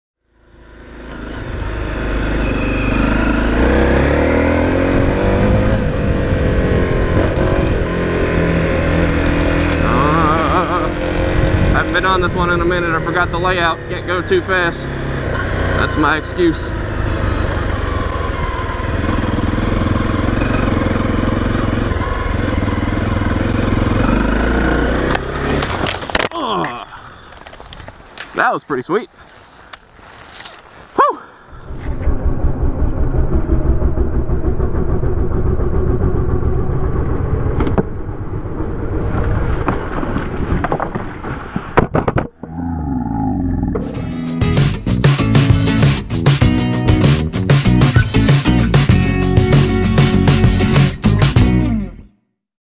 Honda CRF250L Crash!
Quick clip of a low side crash on my Honda CRF250L from a ride in the woods today! Full Video coming later!

My Bike: 2014 Honda CRF250L

Camera: GoPro Hero 3+ Black – Almost always shot in 1080P/60FPS/Wide

Helmet: Shoei Hornet X2 w/ Dark Smoke Sheild and Sena SMH10R Bluetooth

Jacket: Alpinestars Andes Drystar Jacket

Pants: Bilt Iron Workers Kevlar Jeans

Boots: Forma Terrain TX W/ HPS Sole (3 Season)

Gloves: Fox Racing Bomber Gloves

Thank you so much again for your support!